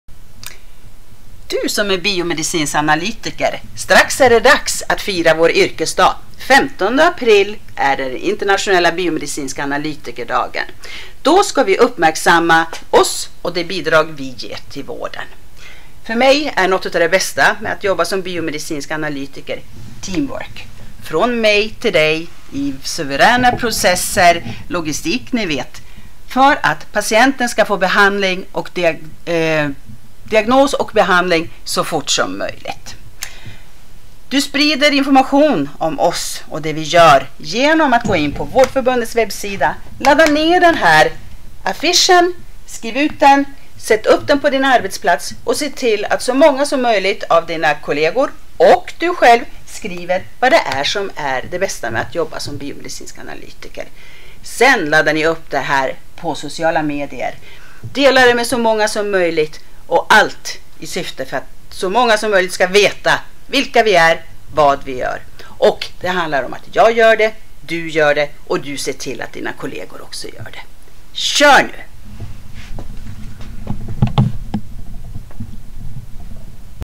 Engagera dig på Biomedicinska analytikerdagen 15 april 2019!
Den 15 april är det den internationella yrkesdagen för  biomedicinska analytiker. Då är det ett ypperligt tillfälle att lyfta yrkesfrågorna och göra yrket mer synligt.
Gör som vår yrkeshandläggare Anne Lindgren Berndt, sätt upp en affisch på din arbetsplats och gå in på vardforbundet.se för att hitta fler sätt att lyfta yrkesfrågorna.